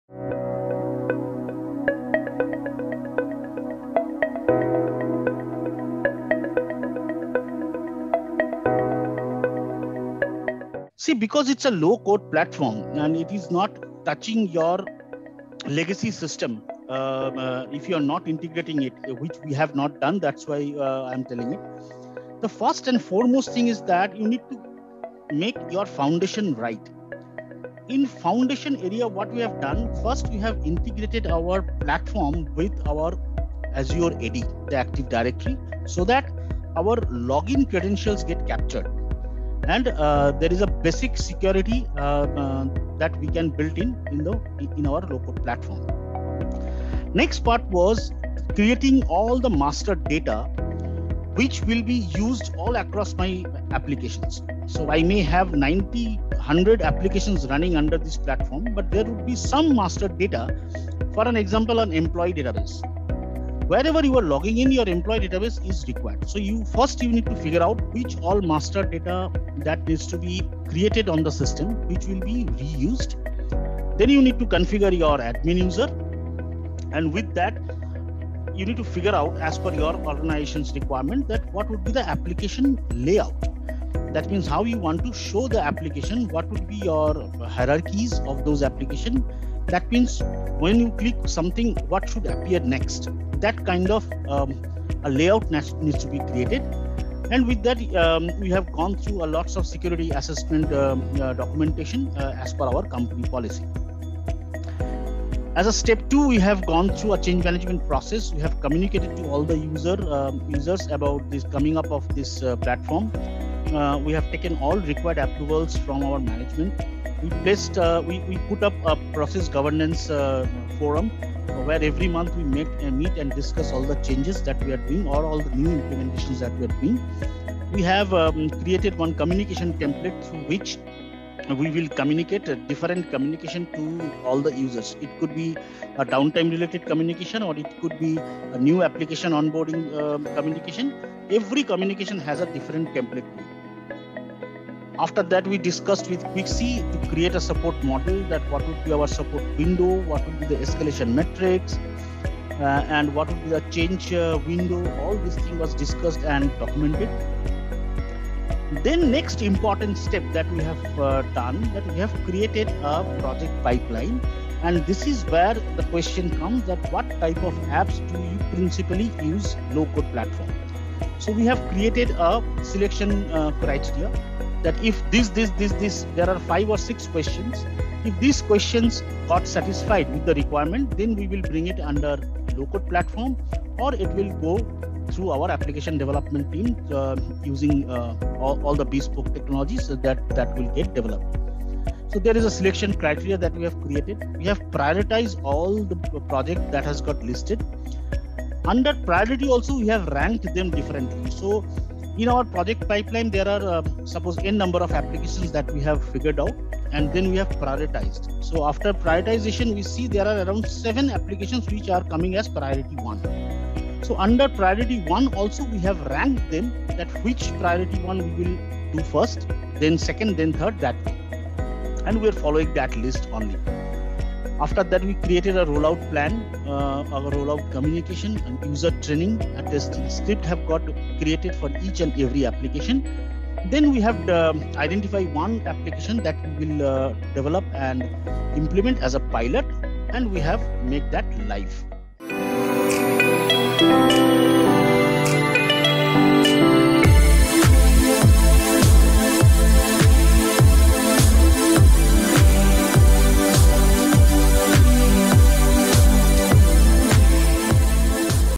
Expert Opinion | What strategy should you use to implement no-code low-code within the organisation?
Don't miss out on this valuable opportunity to hear from expert Rahul Pratanabish on the strategic considerations for implementing lowcode and nocode technologies in your organization.

NoCode is a way to accelerate development and increase efficiency, but it's important to have a well-thought-out strategy to ensure successful adoption and long-term benefits.

Listen to the video and get Rahul's expert opinion on the best approach for your business.